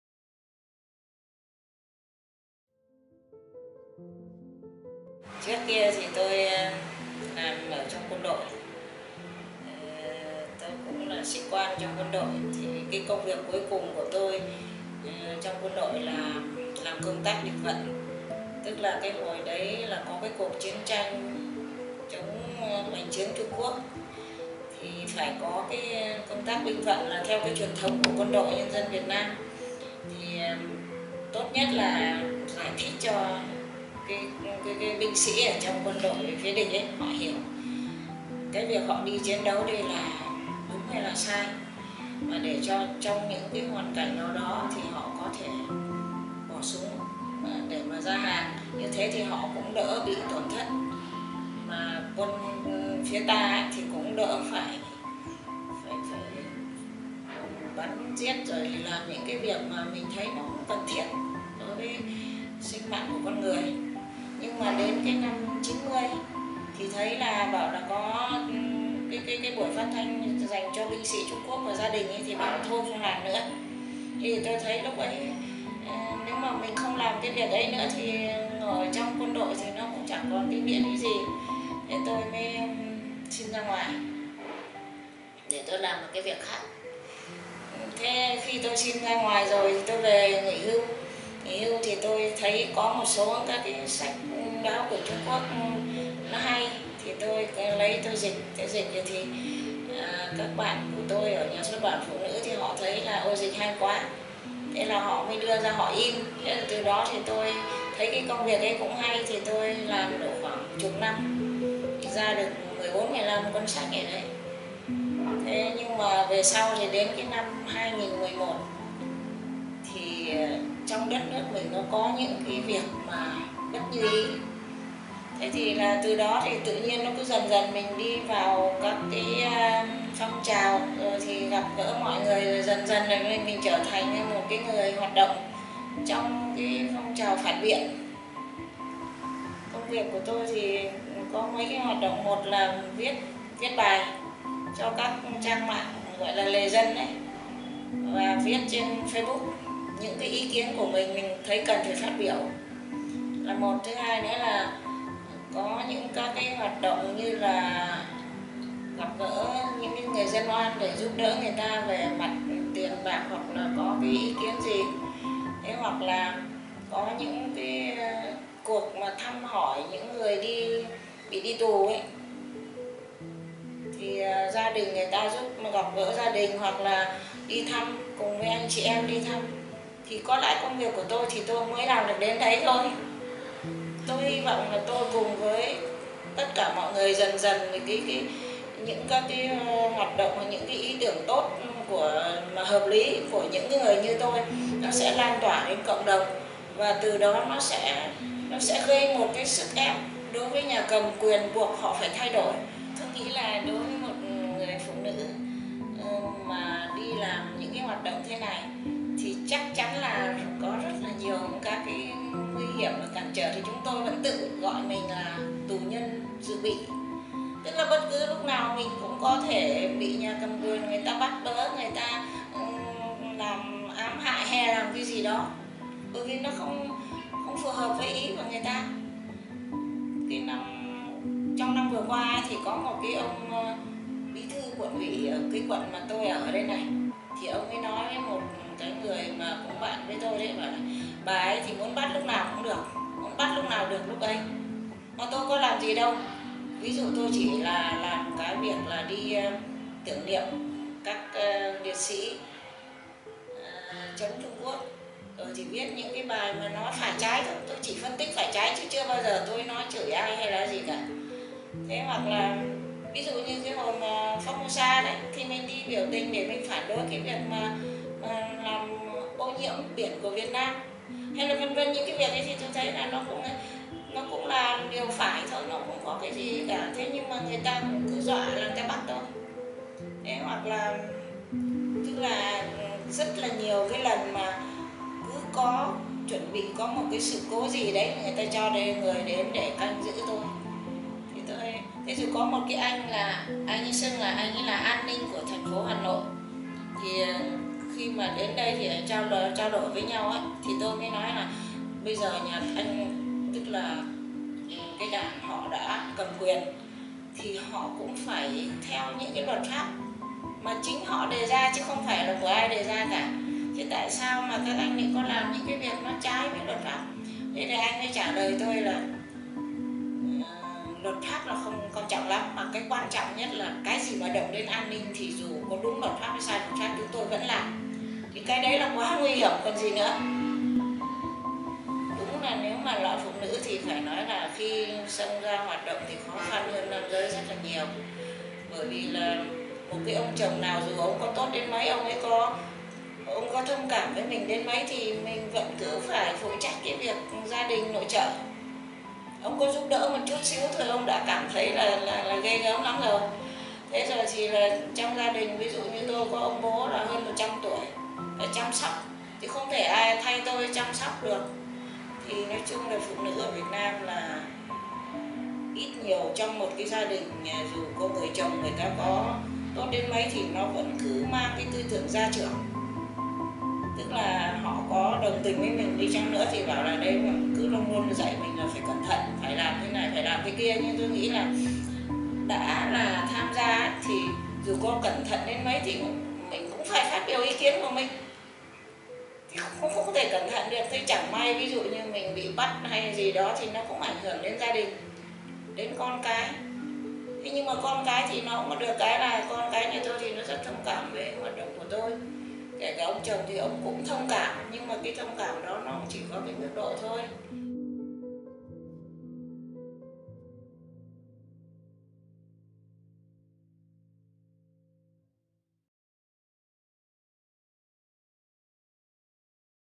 Full interview Nguyen Nguyen Binh
The 4th interview in the series with female activist in Vietnam.
After working in the military, Nguyen Nguyen Binh started translating books. But in 2011, a series of political corruption cases caused her to change path, and she started joining social movements for political change. She spoke to us about what it means for her to be a female activist.